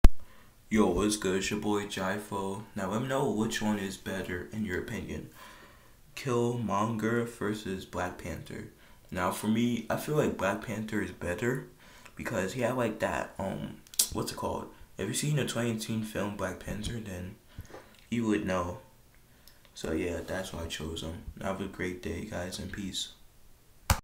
Which one do you think is better? Black Panther vs. Killmonger
I feel like black panther is better